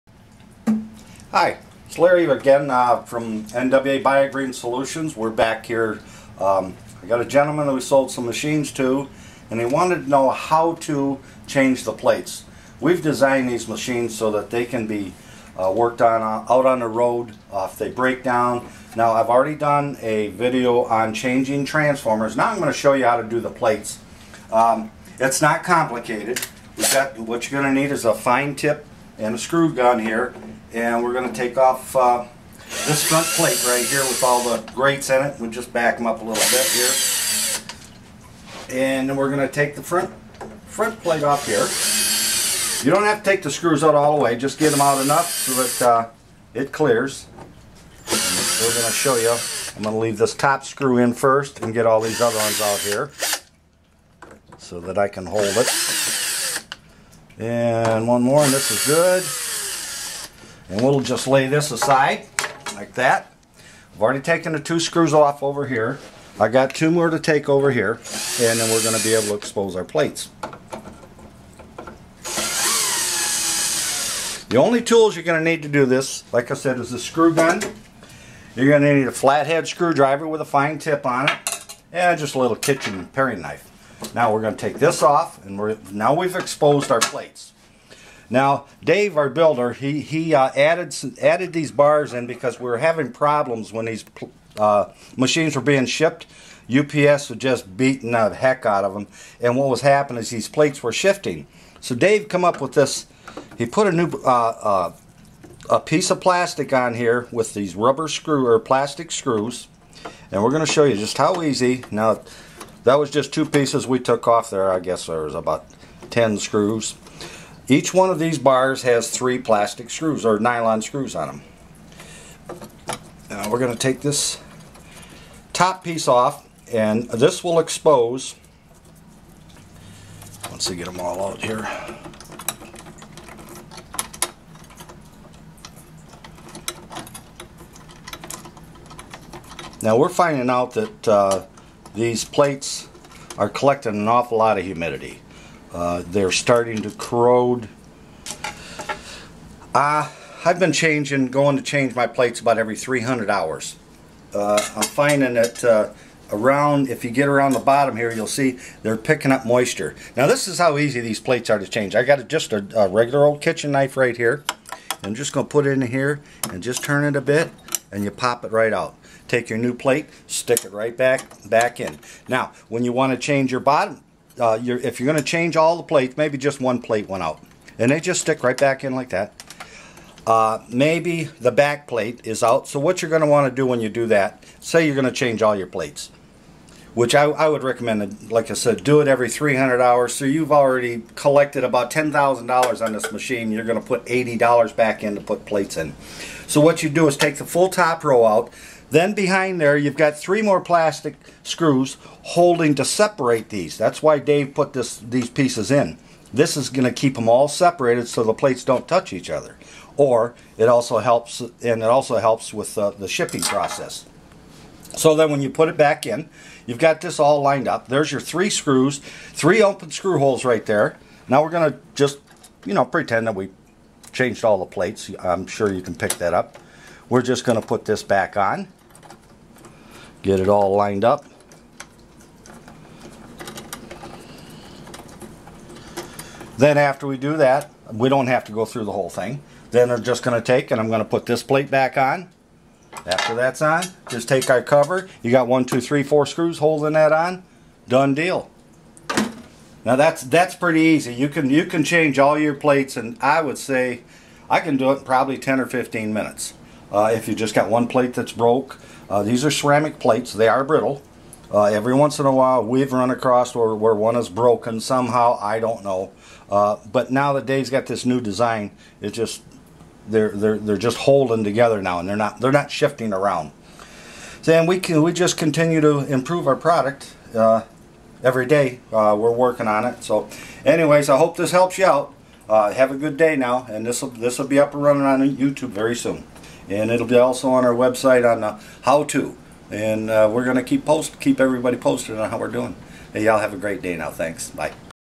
How-To Video on how to change out the electrical plates on our ozone machines
Larie is demonstrating how to easily replace the burnt out or damaged electrical plates on one of our 35,000 mg/ph "Eliminator" ozone machine